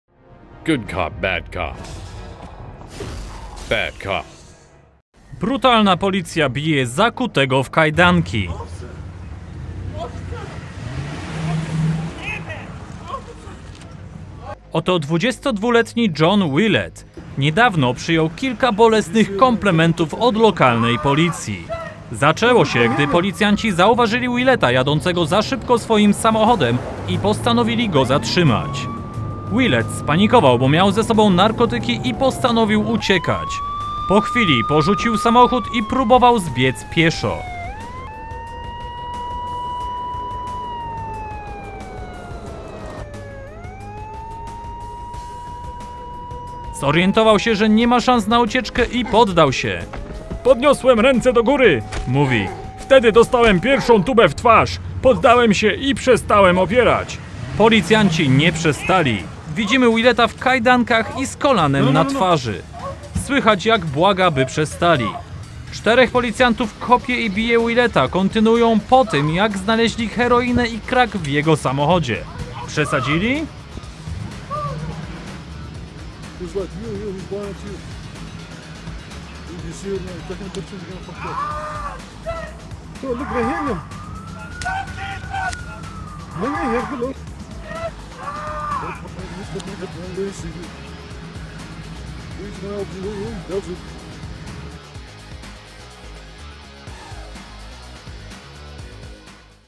Brutalna policja bije zakutego w kajdanki
To 22­letni John Willet

Niedawno przyjął kilka bolesnych komplementów od lokalnej policji.
Zaczęło się, gdy policjanci zauważyli Willeta jadącego za szybko swoim samochodem i postanowili go zatrzymać.

Willet spanikował bo miał ze sobą narkotyki i postanowił uciekać.

Po chwili porzucił samochód  i próbował zbiec pieszo.

Zorientował się, że nie ma szans na ucieczkę i poddał się.

„Podniosłem ręce do góry", mówi „Wtedy dostałem pierwszą tubę w twarz. Poddałem się i przestałem opierać"

Policjanci nie przestali. Widzimy jak Willeta w kajdankach i z kolanem na twarzy. Słychać jak błaga by przestali.
Czterech policjantów kopie i bije Willeta, kontynuują po tym jak znaleźli heroinę i crack w jego samochodzie.

Przesadzili?